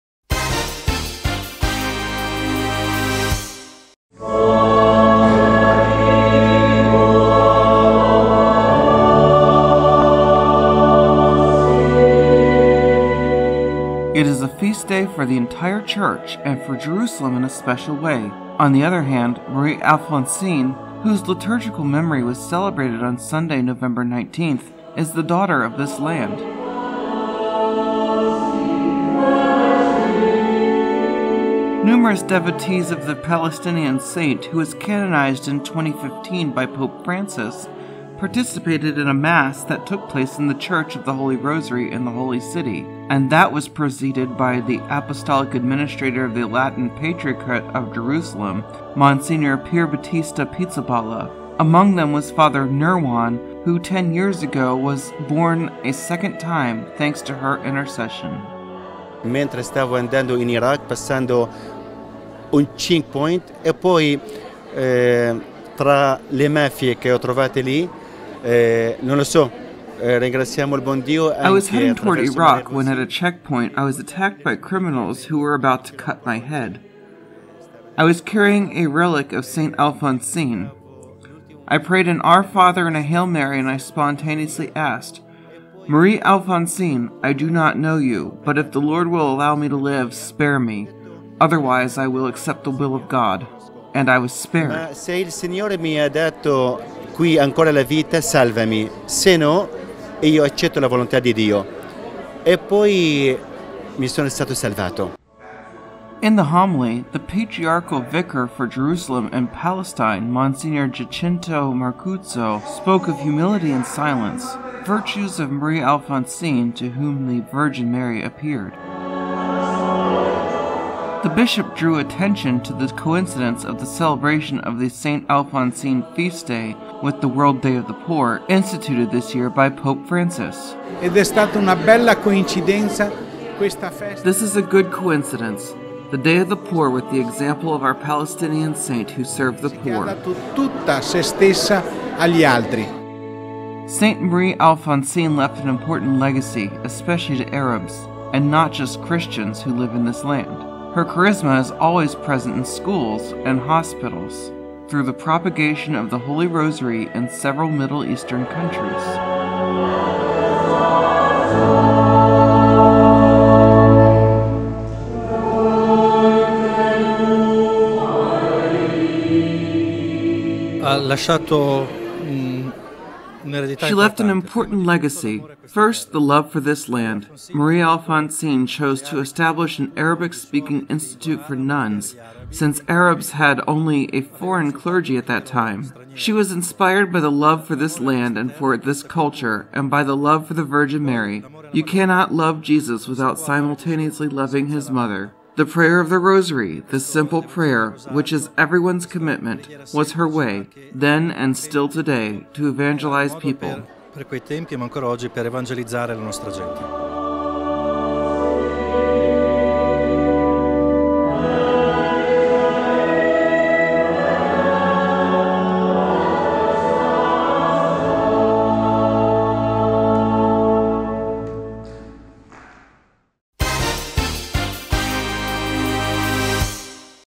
Celebration of Anniversary of Marie Alphonsine and the World Day of the Poor
On Sunday, November 19, the Church celebrated the liturgical commemoration of St. Marie Alphonsine. The celebration coincided with the First World Day of the Poor, instituted by Pope Francis.